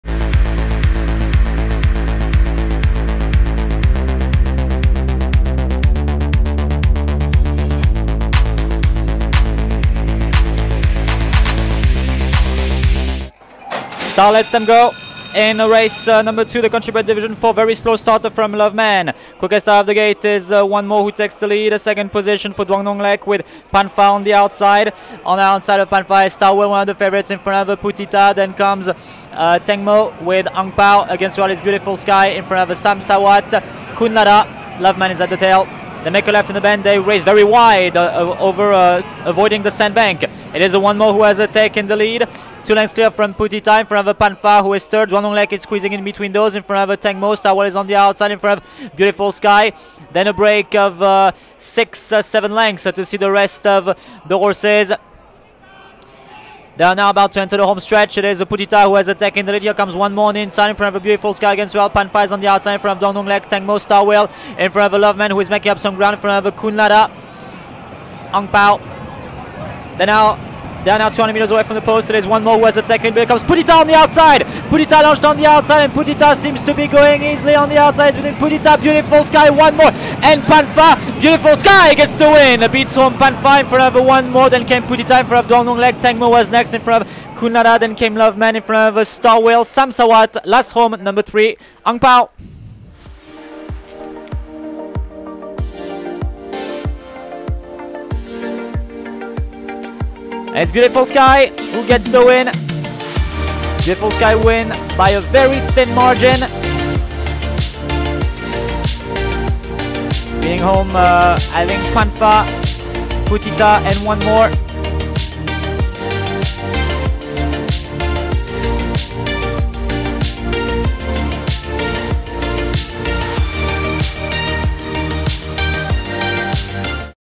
BEAUTIFUL SKY wins! - 27/10/2024 - Race 2 - Country Bred Division 4
Bangkok, Thailand - BEAUTIFUL SKY wins! - 27/10/2024 - Race 2 - Country Bred Division 4

Track Going - Good To Soft